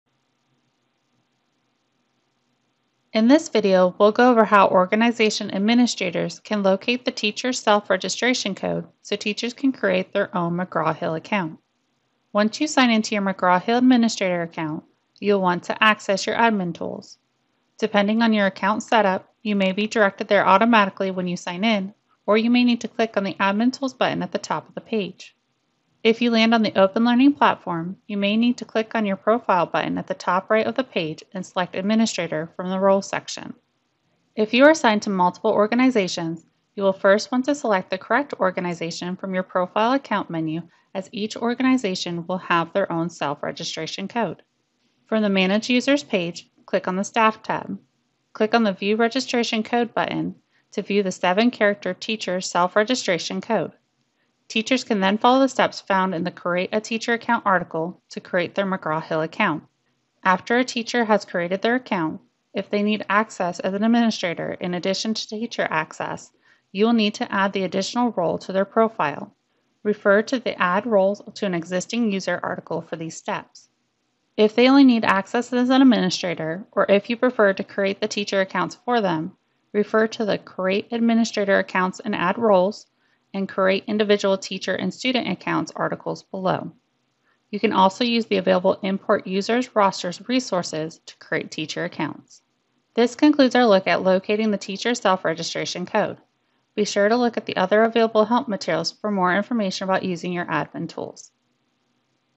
Locate Teacher Self-Registration Code
In this video we’ll go over how Organization Administrators can locate the teacher self-registration code so teachers can create their own McGraw Hill account.